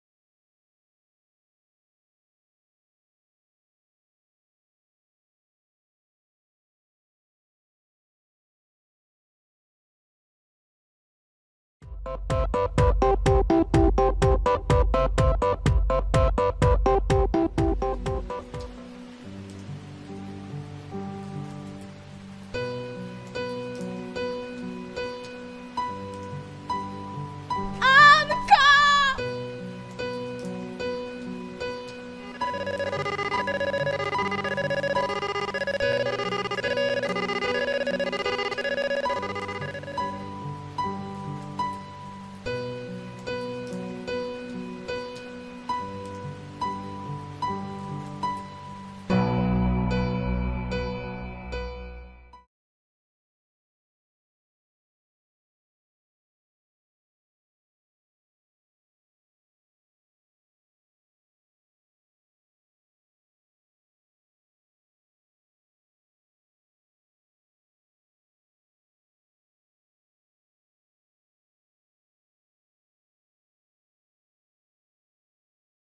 Nie idź moim śladem - ZSP Stąporków
Spot filmowy "NIE IDŹ MOIM ŚLADEM" przygotowany przez uczniów Zespołu Szkół Ponadgimnazjalnych im. Stanisława Staszica w Stąporkowie do konkursu promującego zdrowy tryb życia , wolny od używek, w tym przede wszystkim od narkotyków i tzw. dopalaczy pt. "PRZYTOMNI".